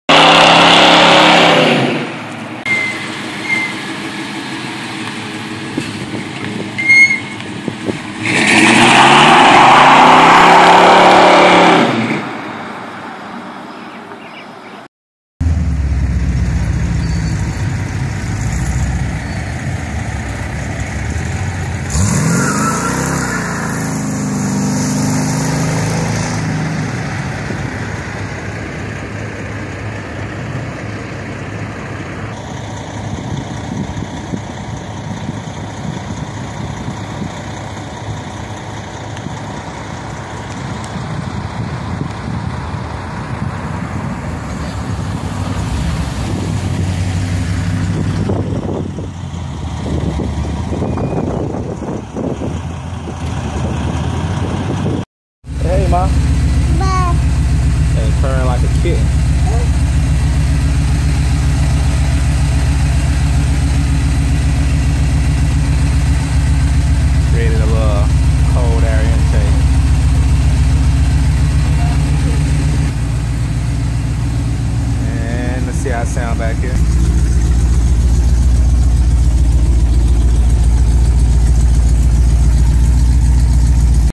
Chevy truck hard on the accelarator Burning rubber!
my truck laying down rubber and and a brief look at the truck.